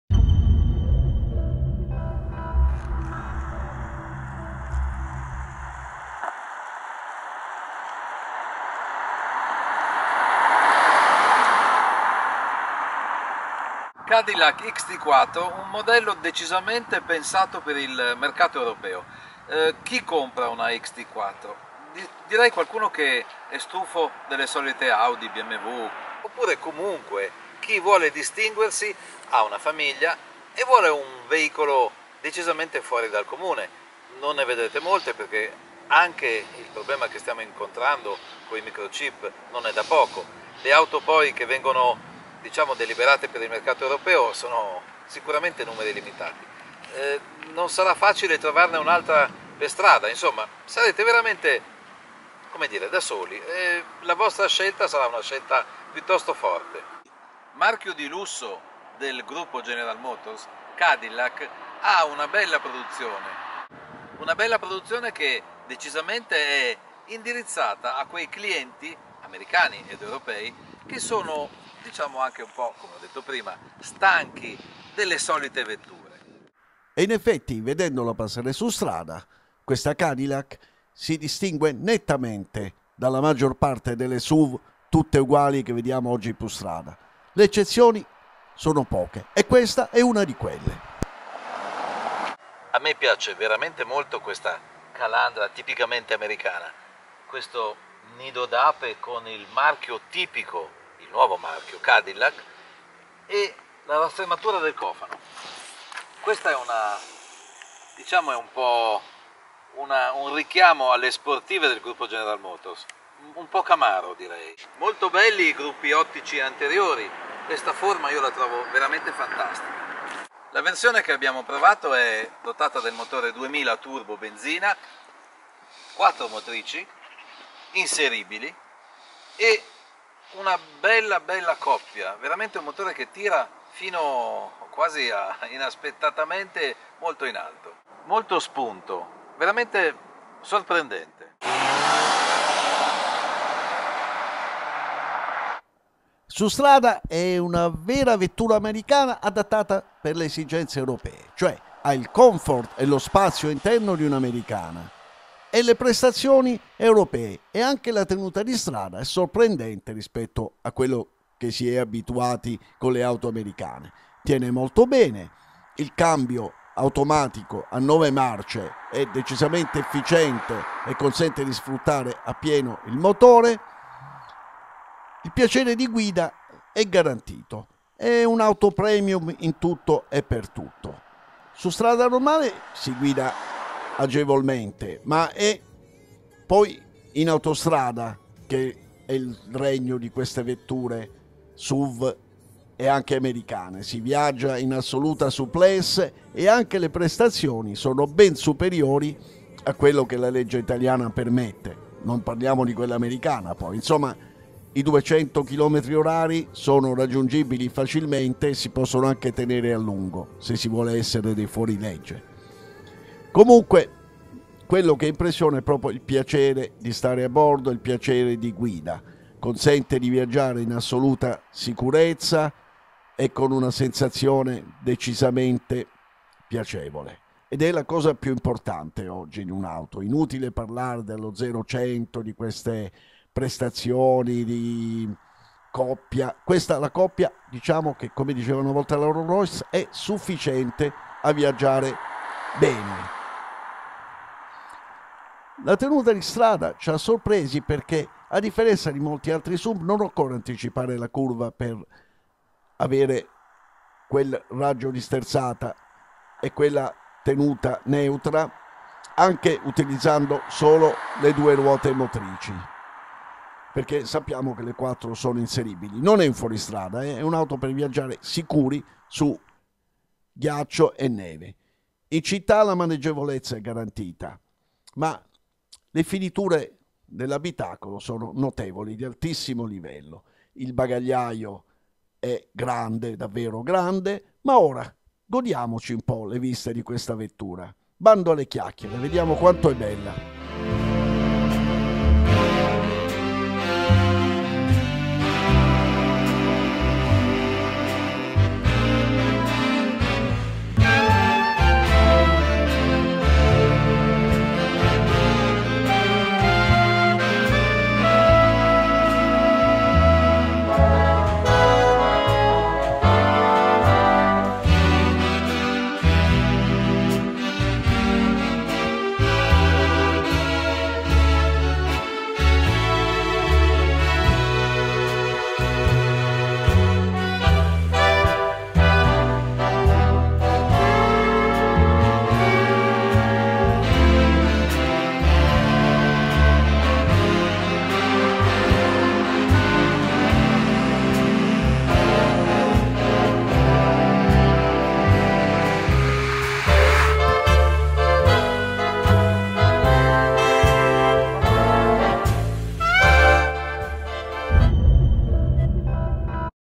CADILLAC XT4 TEST DRIVE
La Cadillac XT4 è il SUV compatto premium, con il quale il marchio Americano premium ha debuttato in questo segmento. E’ stata progettata per lo scenario europeo e un target di automobilisti esigenti, attenti alla qualità e all’innovazione, che vogliono distinguersi
Auto Digest TV ha provato la Cadillac XT4 con il quattro cilindri 2.0 Turbo a benzina, che si aggiunge alla Diesel, e che dispone dell’ innovativo “tripower system progettato per ottimizzare prestazioni ed efficienza con modalità operative distinte, tra cui  la disattivazione di un cilindro quando non serva la potenza. Il motore eroga 230 CV (169 kW) a 5.000 giri/min e 350 Nm disponibile da 1.500 a 4.000 giri/min. La curva di coppia quasi piatta, garantisce alla XT4 una sorprendente reattività a tutti i regimi. Il cambio è doppia frizione automatico a 9 marce paddles  al volante. La trazione integrale con possibilità di escludere le ruote posteriori per ottimizzare i consumi. Dotazioni di comfort e di sicurezza decisamente premium: Wireless Apple CarPlay3 e Android Auto4, riconoscimento segnaletica, mantenimento careggiata, frenata assistita, controllo area di retromarcia, retrocamera e automatic park assist.
In Italia la Cadillac XT4 è distribuita dal Gruppo CAVAUTO.
Salite a bordo della Cadilla XT4 con noi per apprezzarne il comfort, le prestazioni e il consumo medio di oltre 10 Km/litro con una guida brillante.